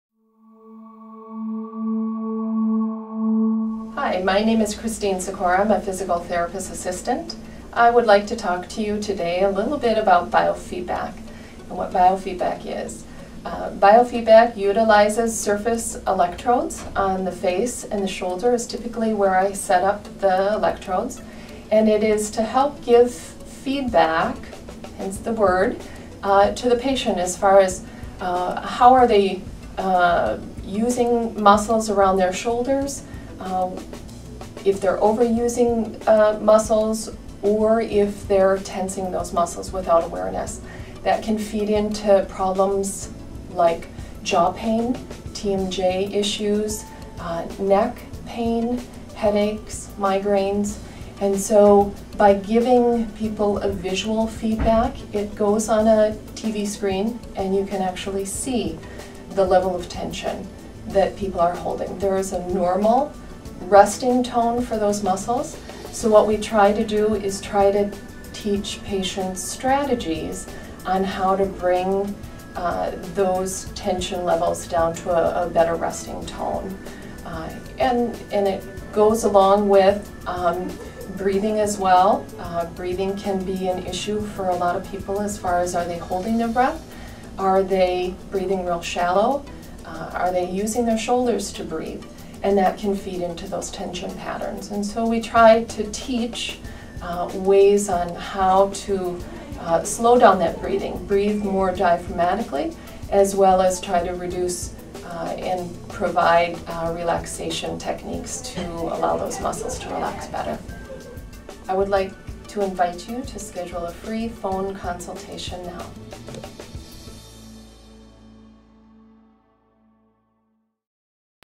What is BioFeedback? Biofeedback for stress related health problems
Here's a video from Kristine Sicora one of OSI's Biofeedback specialists discussing what it is and what to expect from Biofeedback at OSI Physical Therapy.